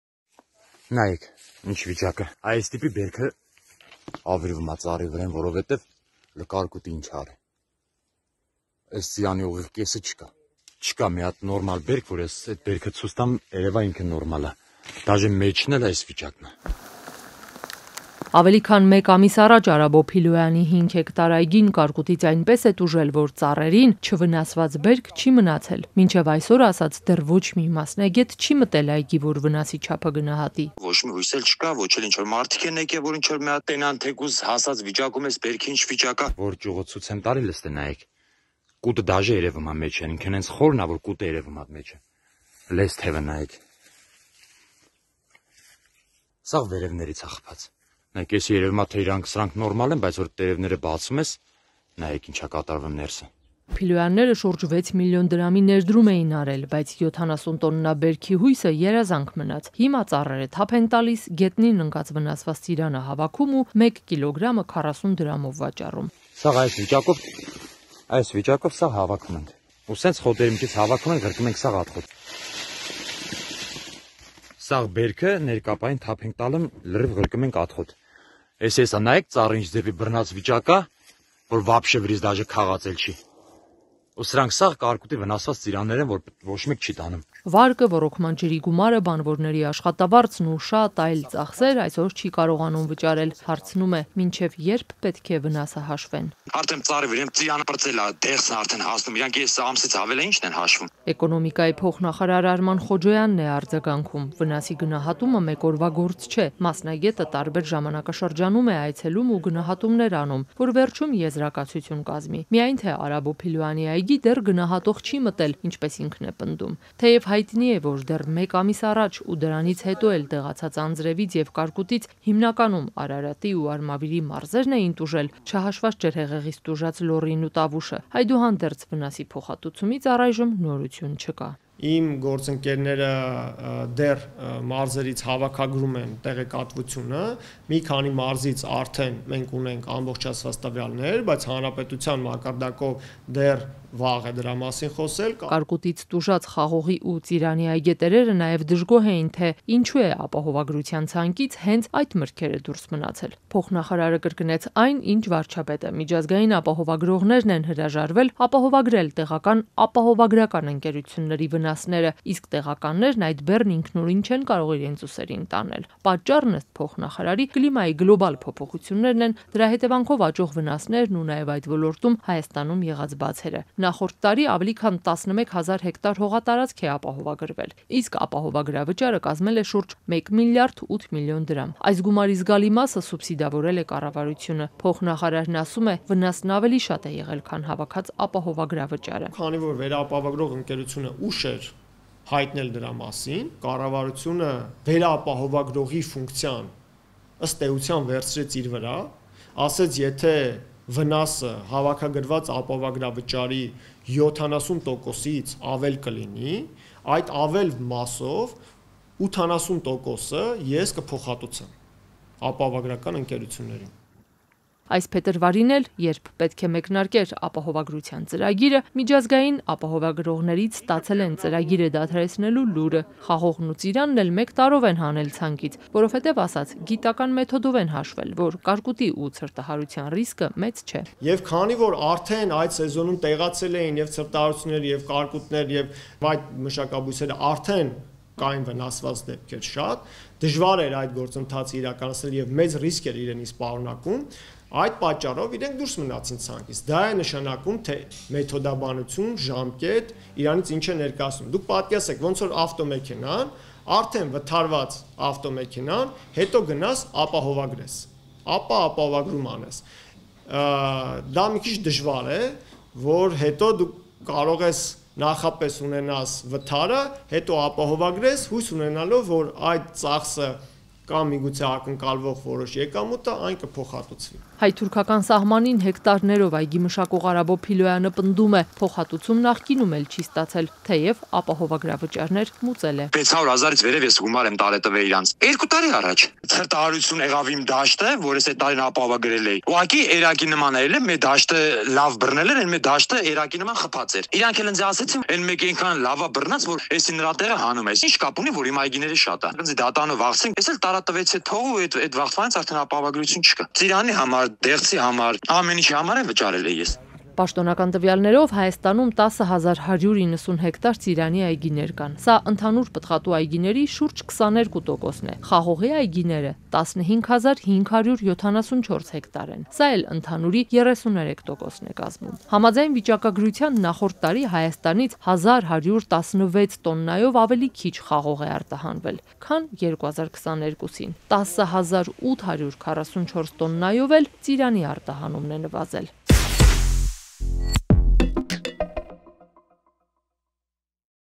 Այգետերը դժգոհ է․ կարկուտից հետո չեն եկել և հաշվել վնասի չափը, փոխնախարարը արձագանքում է
Ավելի քան մեկ ամիս առաջ Արաբո Փիլոյանի 5 հեկտար այգին կարկուտից այնպես է տուժել, որ ծառերին չվնասված բերք չի մնացել: Մինչև այսօր, ասաց, դեռ ոչ մի մասնագետ չի մտել այգի, որ վնասի չափը գնահատի։

Փիլոյանները շուրջ 6 մլն դրամի ներդրում էին արել, բայց 70 տոննա բերքի հույսը երազանք մնաց: Հիմա ծառերը թափ են տալիս, գետնին ընկած վնասված ծիրանը հավաքում ու 1 կիլոգրամը 40 դրամով վաճառում. - «2 օր տվեցինք 60 դրամով, 50 դրամով, հետո իջացրին 40 դրամ»: